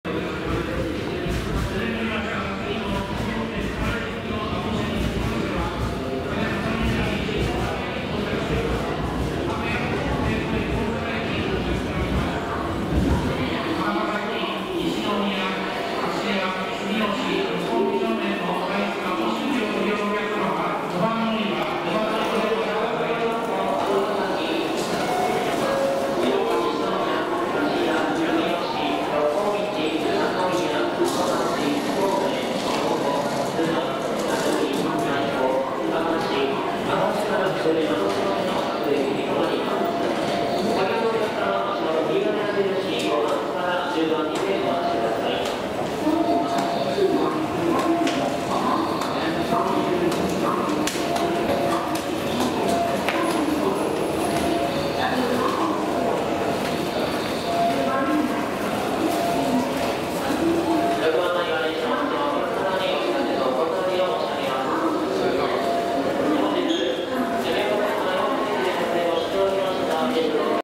2019年1月26日 大津駅と藤阪駅で人身事故発生！③ 動画は大阪駅の案内表示
2019年1月26日13時30分頃に大津駅で人身事故が発生しました。琵琶湖線の運転については発生から1時間後に再開しましたが、16時27分頃に学研都市線の藤阪駅で人身事故が発生しました。

更新状況についてはホームページ・ブログ・Twitterで確認してください。